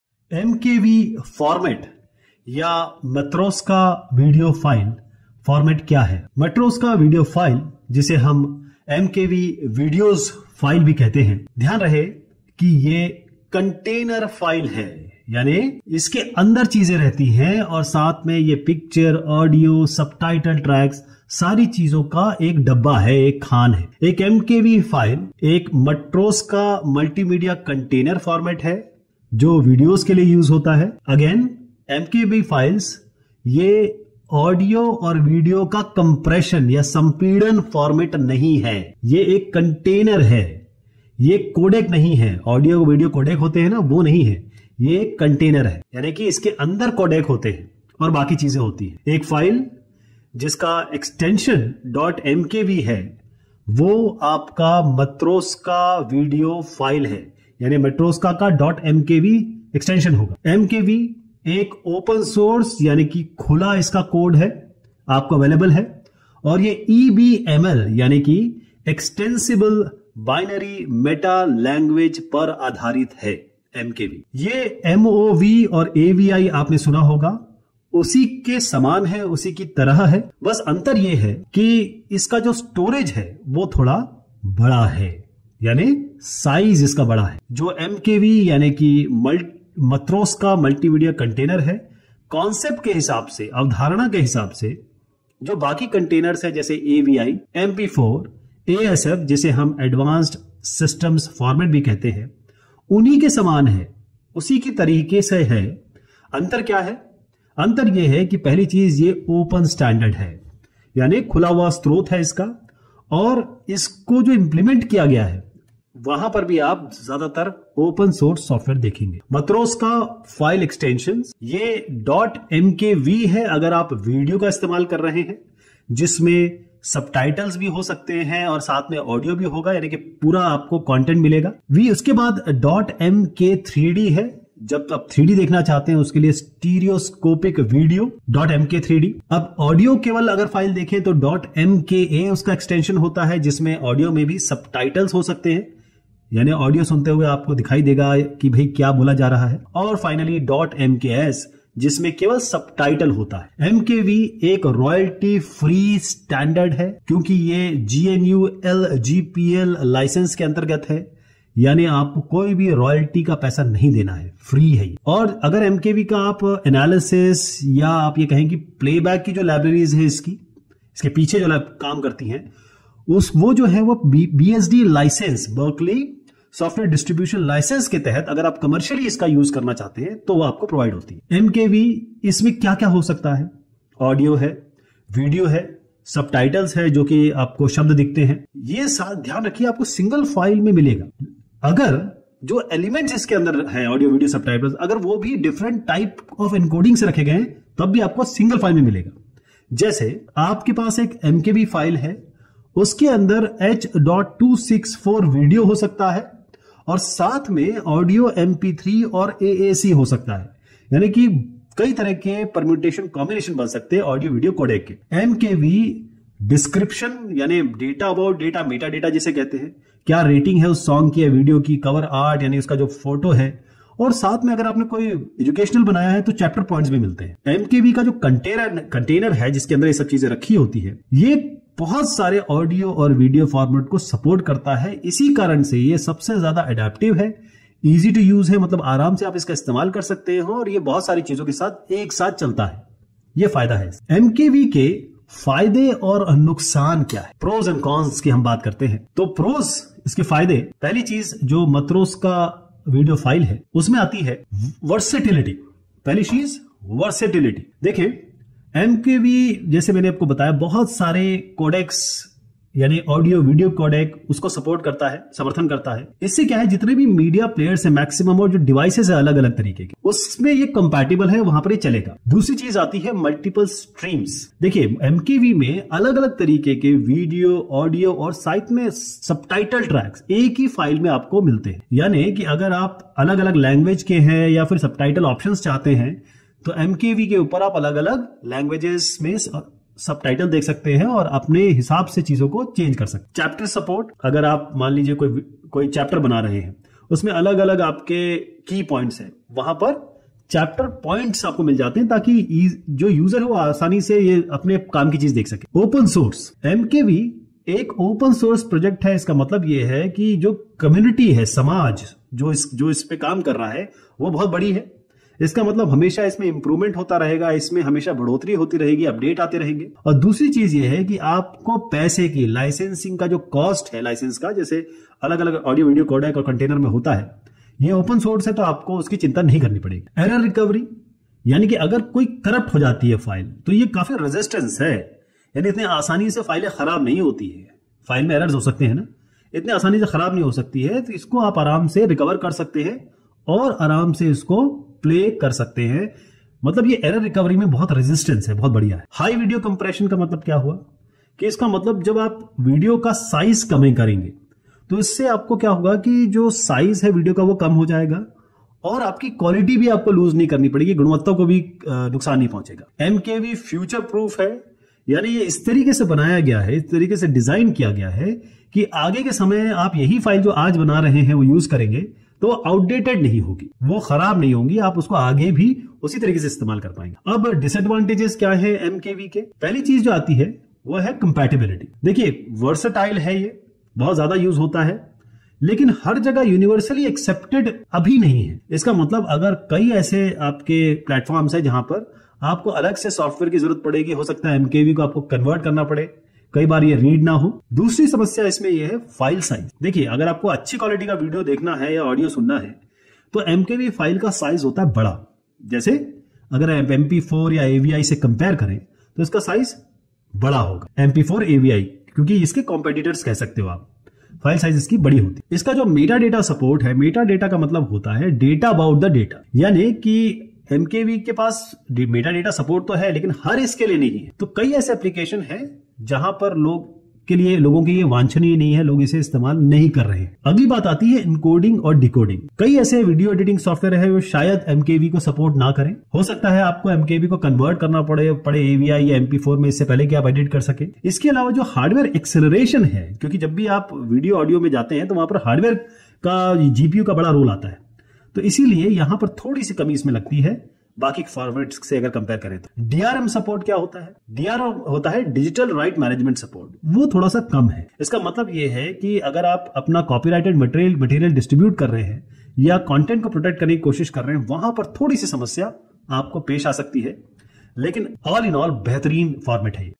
MKV Format Explained|What is MKV Format (Matroska Video File)?|Pros & Cons of MKV Format HINDI URDU
What Is MKV Format (Matroska Video File)?
Matroska Video files, also known as MKV videos, are container files that support unlimited picture, audio, and subtitle tracks.
An MKV file is the Matroska multimedia container format used for video.
MKV files are not an audio or video compression format.
MKV is a container and not a codec.
A file with the .MKV file extension is a Matroska video file.
MKV is an open-source format based on the EBML (Extensible Binary Meta Language).
They're similar to MOV and AVI files, except they have a much larger amount of storage.
The Matroska Multimedia Container is similar in concept to other containers like AVI, MP4, or Advanced Systems Format (ASF), but is an open standard and its implementation consists mostly of open source software.
Matroska file extensions are .mkv for video (which may include subtitles or audio), .mk3d for stereoscopic video, .mka for audio-only files (which may include subtitles), and .mks for subtitles only.
MKV analysis and playback libraries are provided under the BSD license for commercial enterprise use.
MKV can incorporate audio, video, and subtitles in a single file even if those elements use different types of encoding.
For example, you could have an MKV file that contains H.264 video and MP3 or AAC for audio.
MKV also supports descriptions, ratings, cover art, and even chapter points.
The MKV container itself also supports almost any audio and video format, making the format highly adaptive and easy to use.
MKV Pros and Cons
Pros of MKV:
• Versatility: MKV supports a wide range of codecs, making it compatible with various media players and devices.
• Multiple streams: It can contain multiple video, audio, and subtitle tracks in a single file, making it convenient for users who prefer different languages or subtitle options.
• Chapter support: The format supports chapter points, allowing users to easily navigate through the video file.
• Open-source: MKV is an open-source project, which means its development is driven by the community, ensuring continuous improvements and updates. Also, you don't have to worry about the cost of licensing with MKV, because it's open source.
• Error recovery: The format is more resistant to corruption, and in case of file errors, it is usually easier to recover the playable content.
• High Video Compression: It has high video compression rates, enabling you to store large files without losing quality.
• Future Proof: MKV container files were designed to be future proof, meaning that the files would not become outdated.
Cons of MKV:
• Compatibility: Despite its versatility, MKV is not universally supported by all devices and media players, which can sometimes require additional software or conversion.
• File size: High-quality video and audio in MKV files can result in larger file sizes compared to other formats like MP4 or AVI.
• Limited metadata support: MKV does not support extensive metadata as some other formats, which can make it less desirable for certain applications.
• Encoding and decoding: Some video editing software may not support MKV, requiring conversion before editing. Additionally, hardware acceleration for encoding and decoding MKV files may not be as widely supported as other formats.
• Lack of DRM support: MKV does not support digital rights management (DRM) natively, which may limit its use for distributing copyrighted or protected content.

MKV Format Explained,What is MKV Format (Matroska Video File)?,Pros & Cons of MKV Format,